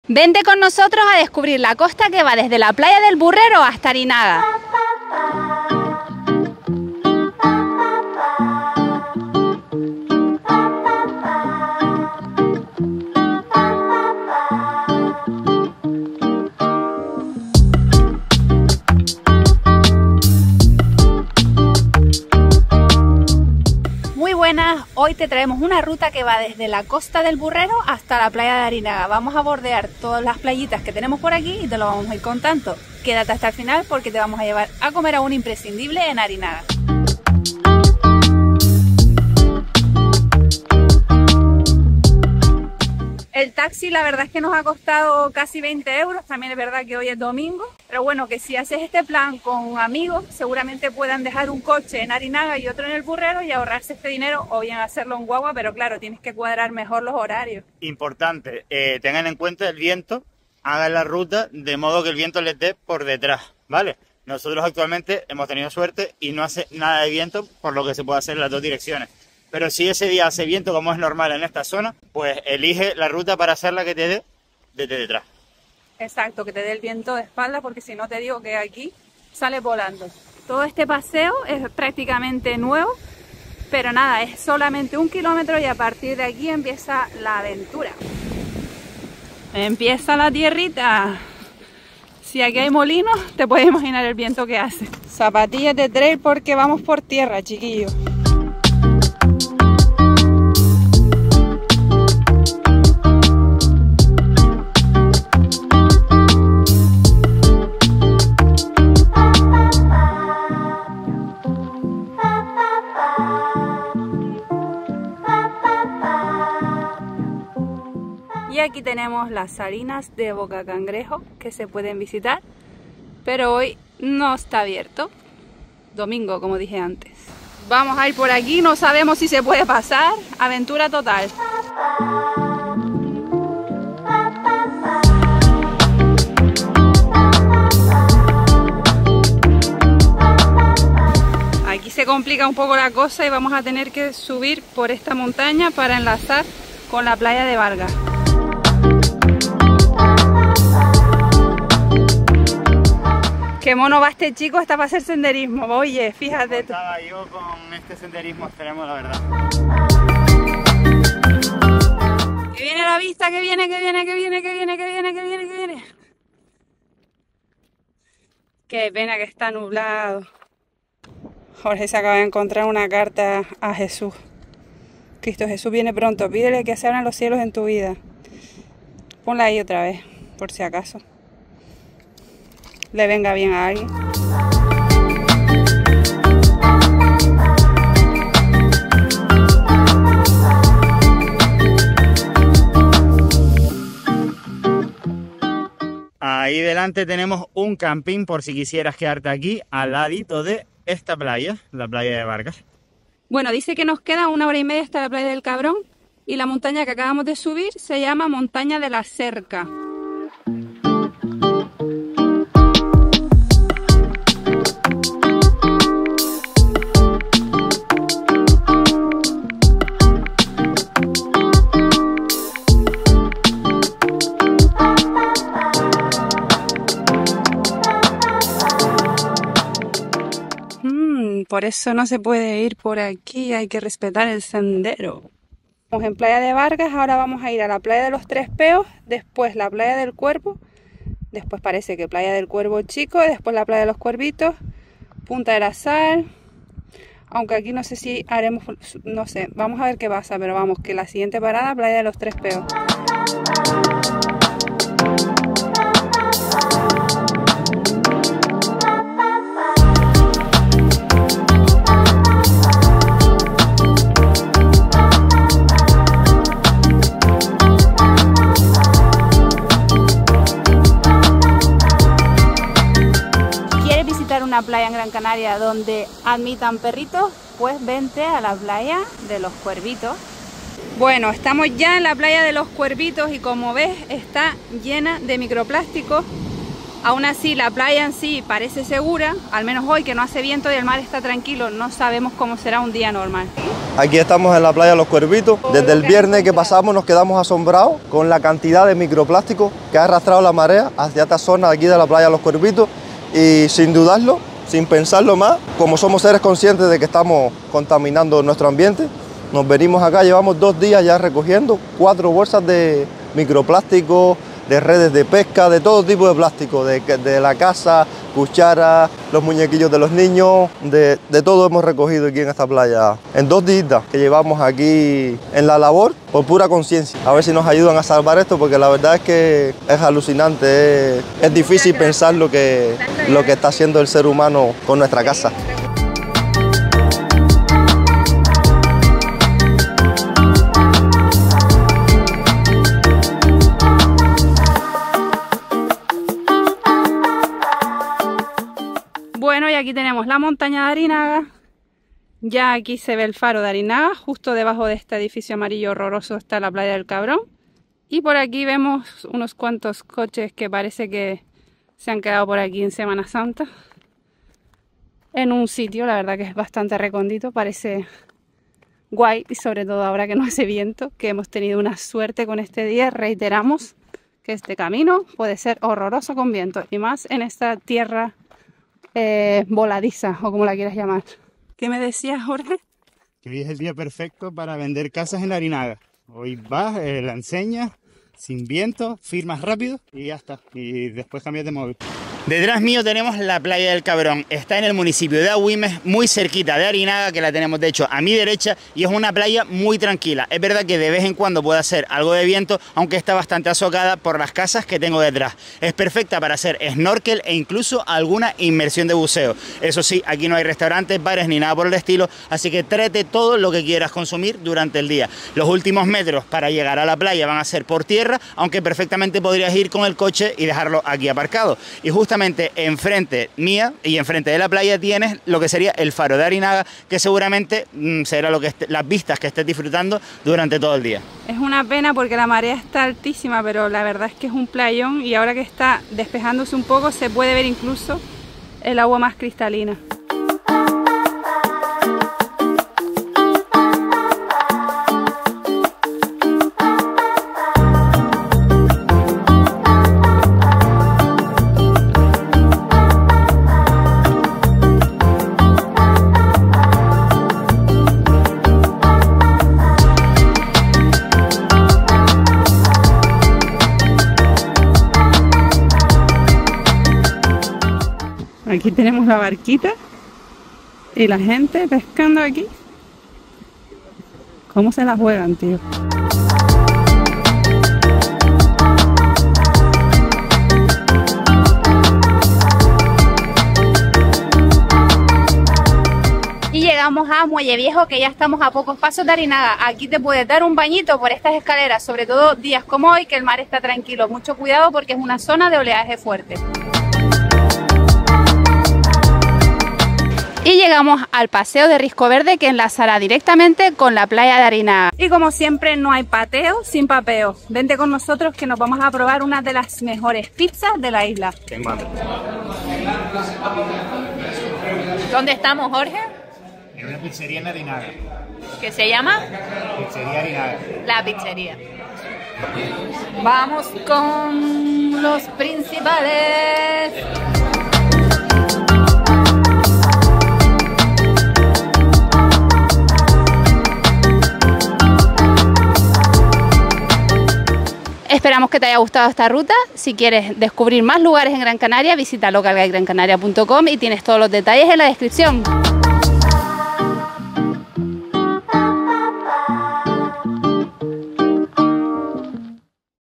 Ruta de SENDERISMO desde El BURRERO hasta ARINAGA por la costa, Gran Canaria
En esta ocasión te llevamos de ruta de senderismo desde El Burrero (Ingenio) hasta Arinaga (Agüimes) en la costa sureste de Gran Canaria. Recorreremos 9 kilómetros por la costa con parada en las salinas de Bocacangrejo, la montaña de La Cerca, playa de Vargas, playa de los Tres Peos, playa de Los Cuervitos, playa del Cabrón, Faro de Arinaga, Muelle Viejo y Risco Verde.

Esta ruta es de nivel intermedio / avanzado y recomendamos llevar al menos 1 litro de agua por persona, calzado de trail, un track y coger un taxi o cualquier medio de transporte para la vuelta.

Los días que haga mucho viento no recomendamos hacer esta ruta o bien hacerla en el sentido del viento para que sea lo más agradable posible.

⏰ Contenidos video caminata El Burrero - Playa de Arinaga:
00:00 Bienvenida
00:22 Playa del Burrero y el nuevo paseo
01:42 Salinas de Bocacangrejo, Gran Canaria
02:42 Playa de Vargas
04:10 Playa de Los Tres Peos (playa pet friendly en Gran Canaria)
04:30 Playa de Los Cuervitos (playa para perros en Gran Canaria)
06:32 Playa del Cabrón, Gran Canaria
07:35 Faro de Arinaga, Agüimes
07:58 Muelle viejo Arinaga
08:21 Risco Verde, Arinaga
08:27 La Pizzería Arinaga,
08:58 Conclusiones video y Playa de Arinaga